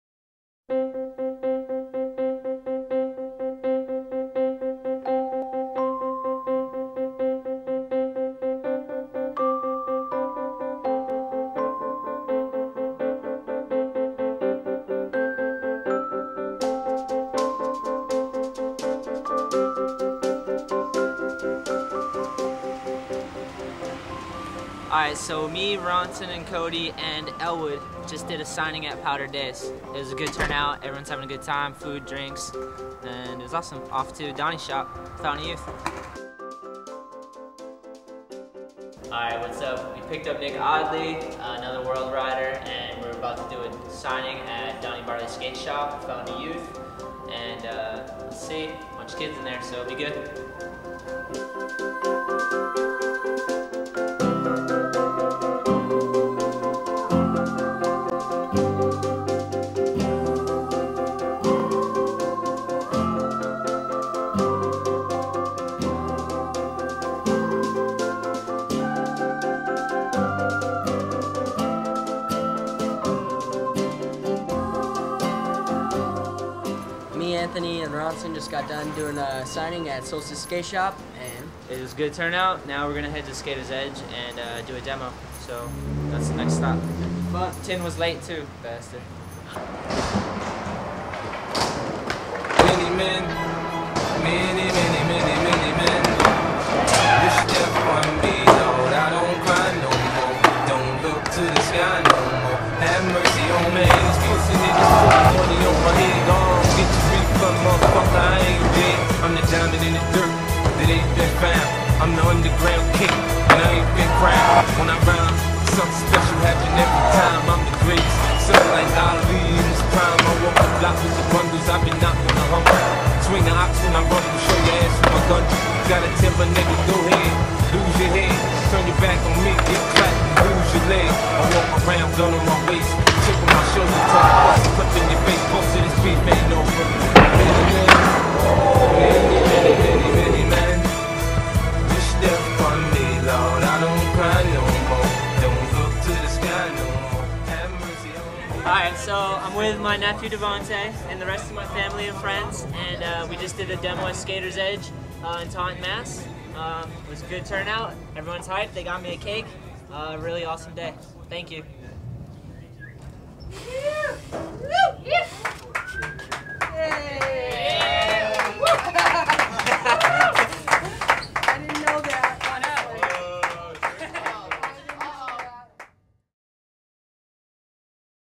Anthony Shetler Board Release wkend
A weekend of signings,demos, and  party in Mass. to celebrate Anthony's first pro model.